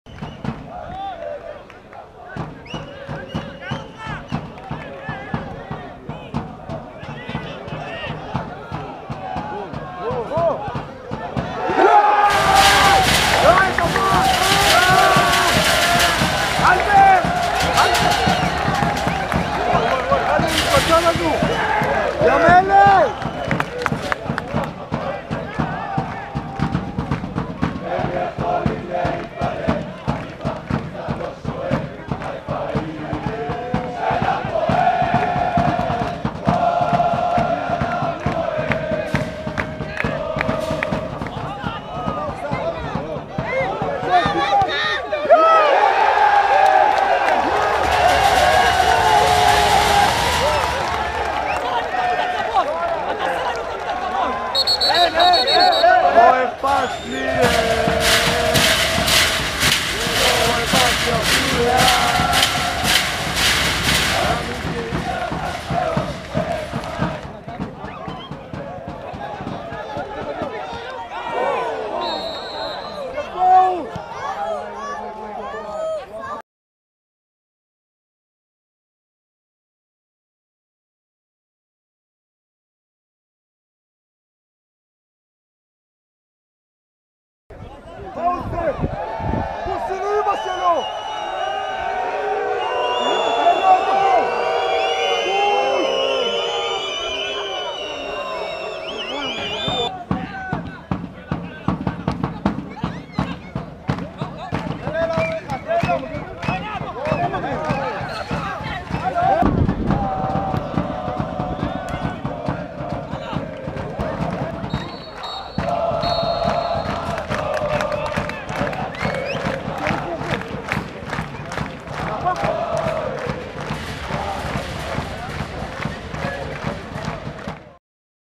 המשחק הראשון בהסטוריה של הפועל רובי חיפה
קטעים נבחרים מהמחצית השנייה של המשחק הראשון בהסטוריה של הפועל חיפה כמועדון בבעלות אוהדים.
הפועל רובי שפירא חיפה 3 - 0 הפועל טירת הכרמל

סיבוב ב' של גביע המדינה, מגרש אבי רן